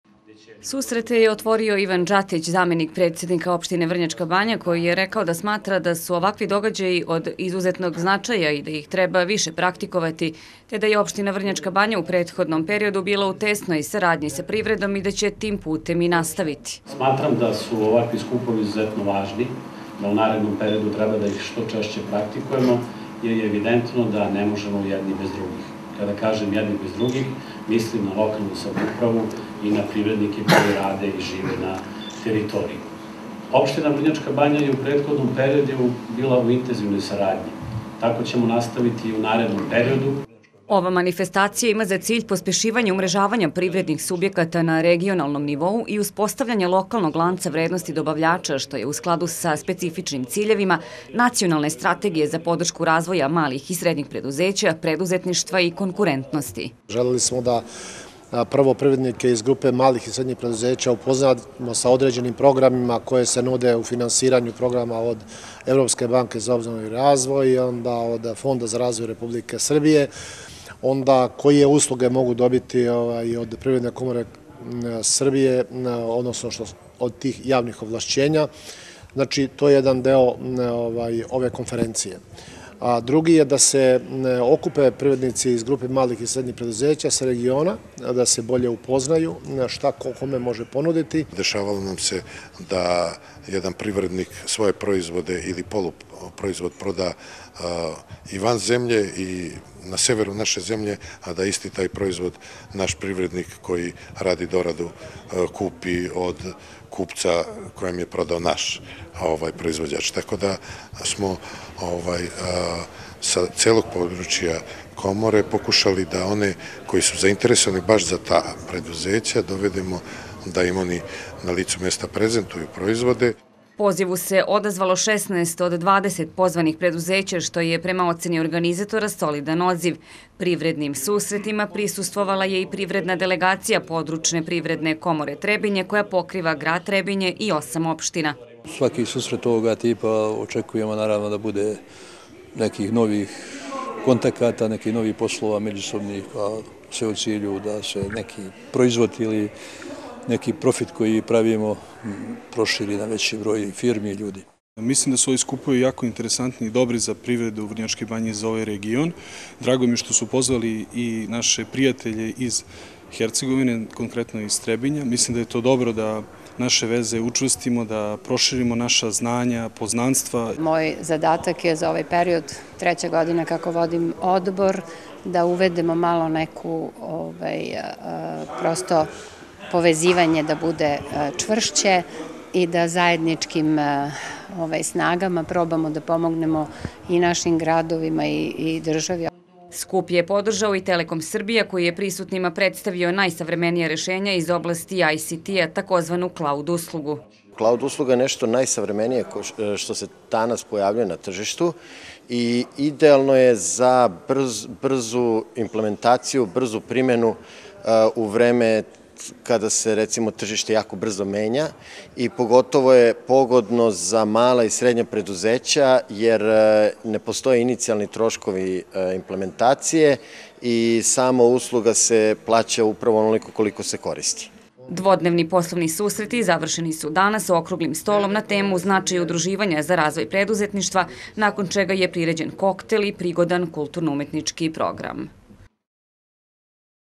TV MELOS KRALJEVO: RPK KRALJEVO POSLOVNI SUSETI U VRNJAČKOJ BANJI
KRALJEVO - Regionalna privredna komora Kraljevo, poslovni susreti u Vrnjačkoj Banji, prilog RTV MELOS

Copyright: Agencija za TV produkciju, web i marketing “On-line”, Kraljevo
Distibution: Informativni portal Kraljevo Online
Directed: Ljubiša Lešević
Camera & Video Edit: Marijan Vučetić
Technical support: Željko Božović